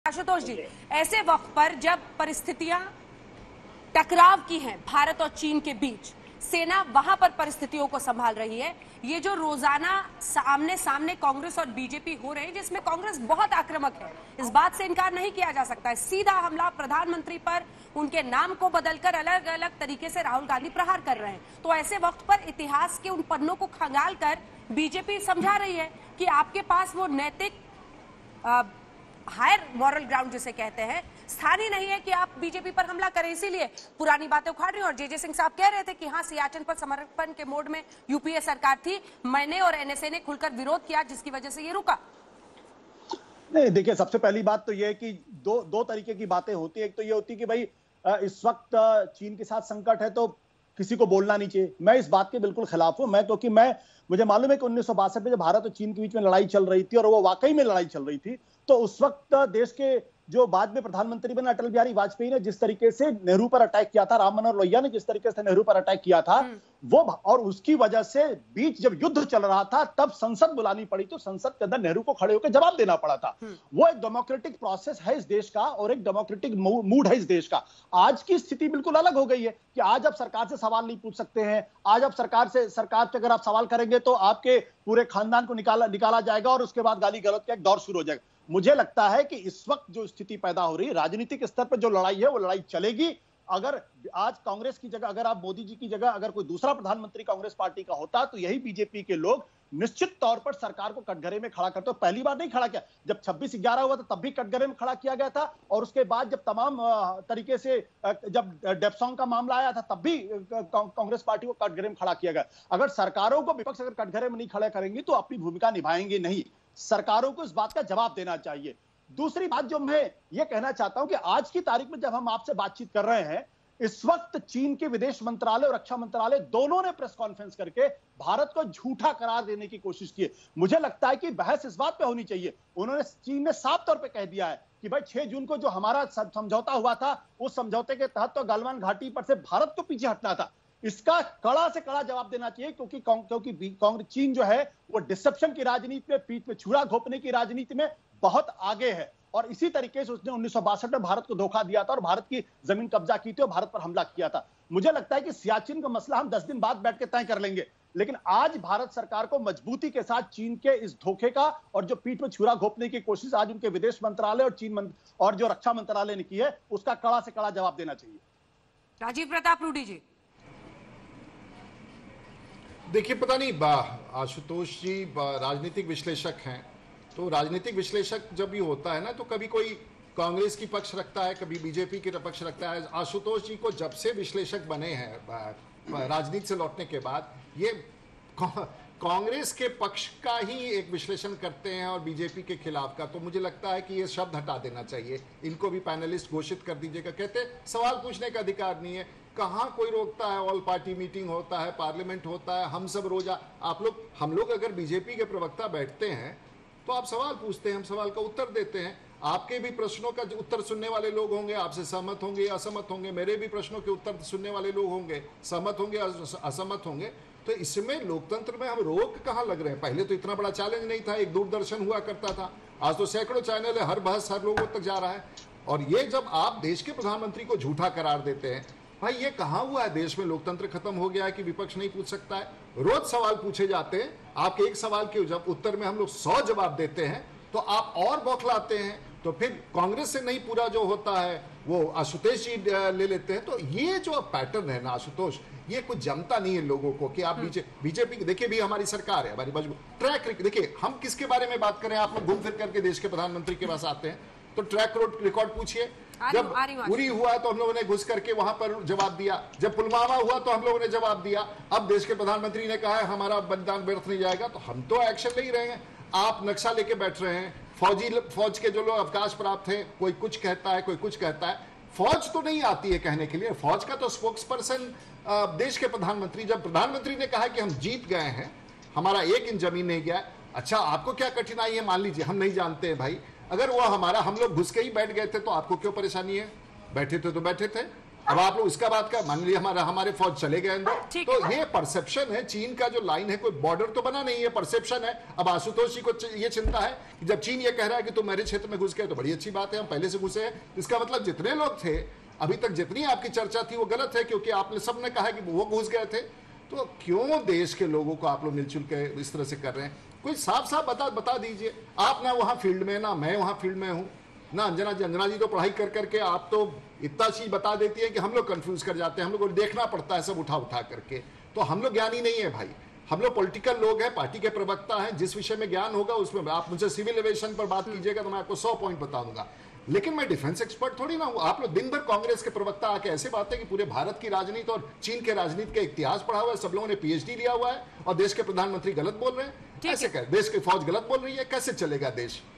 Siachen पर कौन सच्चा कौन झूठा? बोले Ashutosh
ऐसे वक्त पर जब परिस्थितियां टकराव की है. बीजेपी और कांग्रेस आमने सामने हो रहे हैं. इसमें कांग्रेस बहुत आक्रमक है. सीधा हमला प्रधानमंत्री पर. उनके नाम को बदकर अलग-अलग तरीके से प्रहार कर रहे हैं. तो ऐसे वक्ता पर इतिहास के उन पन्नों को खंगालकर बीजेपी समझा रही है और पुरानी बातें सामने रख रही है. इसपर क्या बोले आशुतोष. देखें वीडियो..